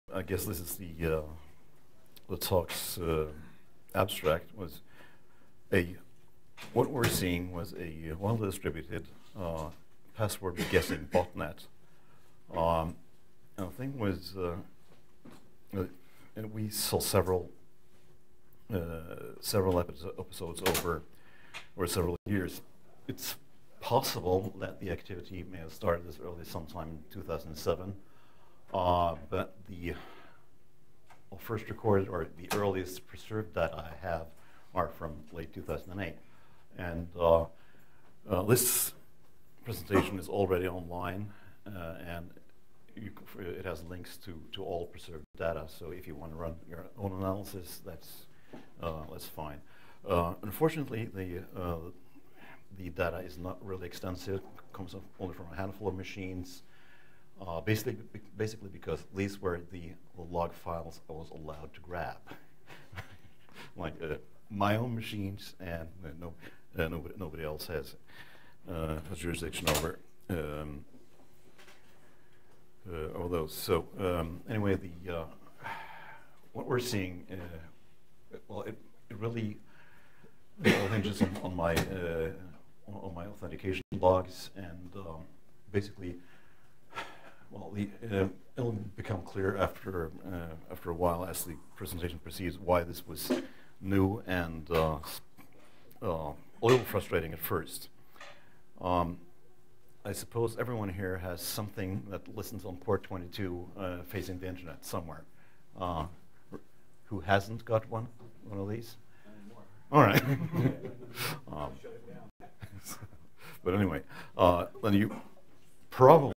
The Hail Mary Cloud And The Lessons Learned
The Future Of Botnets: Low Intensity, Distributed

There was a time when brute force attacks were all rapid-fire and easily blackholed on sight. That changed during the late 2000s: The low intensity, widely distributed password guessing botnet dubbed "The Hail Mary Cloud" that made its debut in 2007 was remarkable for three things:

the service it targeted was SSH, an almost exclusively Unixish-based phenomenon
the glacial pace of attack from each of the participants
the apparent stay-below-the-radar profile
Against ridiculous odds and eventually even some media focus, the botnet apparently thrived for several years. This session presents the known facts as seen by an early observer, proceeds to an analysis of the patterns observed during the various encounters with the phenomenon, with conclusions that may have implications for current detection and prevention strategies and points to remember when formulating future approaches to network security.